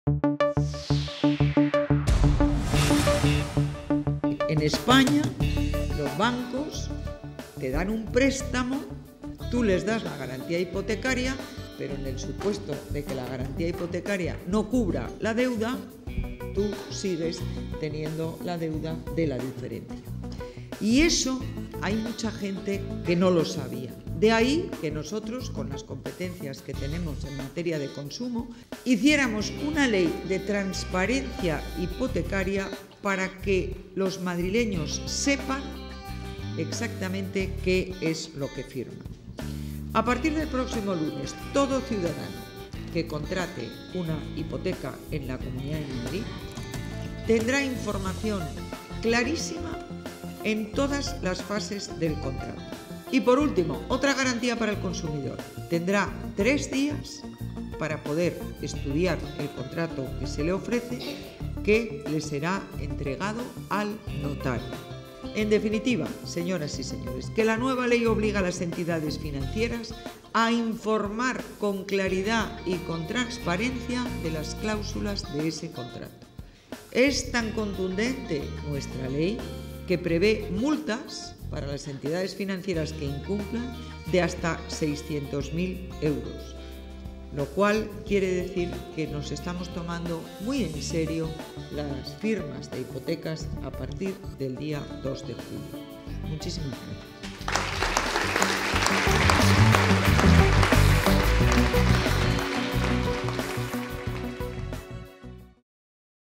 Esperanza Aguirre inaugura la jornada sobre los Préstamos Hipotecarios.
La Presidenta de la Comunidad de Madrid, Esperanza Aguirre, inaugura la Jornada sobre Los Préstamos Hipotecarios. La Ley de Transparencia Hipotecaria de la Comunidad de Madrid en la que participarán representantes de la Administración Pública, de Entidades Financieras, de Órganos de Control Financiero y de Organizaciones Profesionales y Asociaciones de Consumidores.